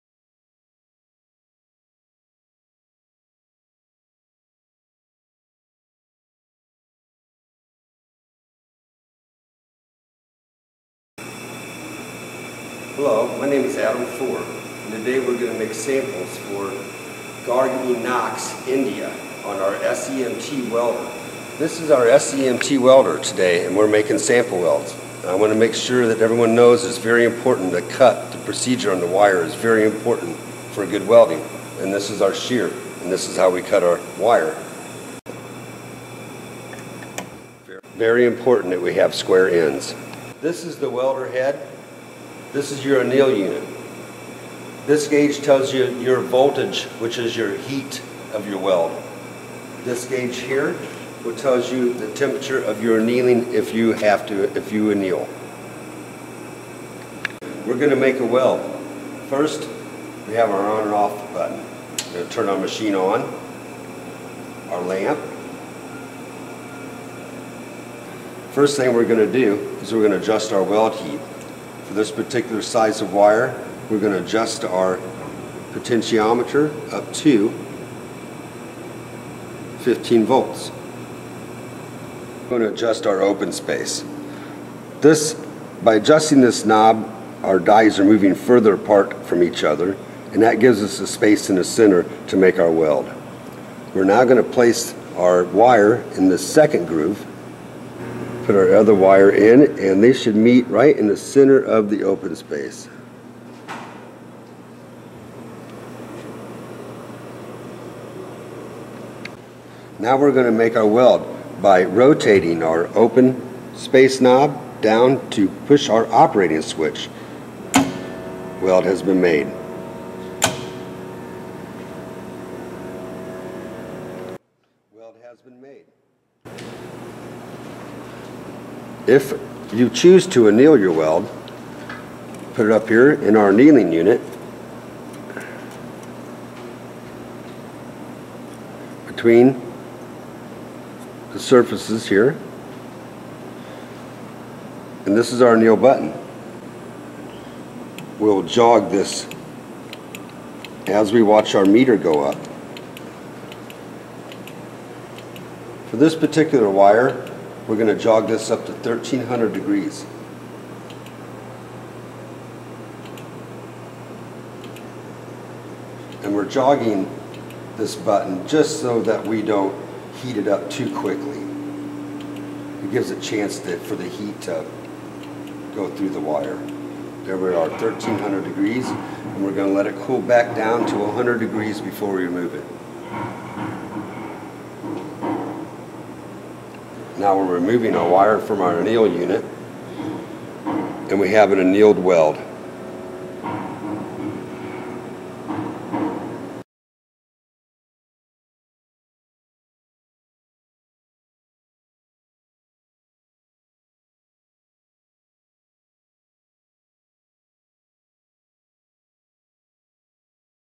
MicroWeld SEMT Demo
Quick Demo of butt welding fine steel wire on Micro-Weld Model SEMT butt welder.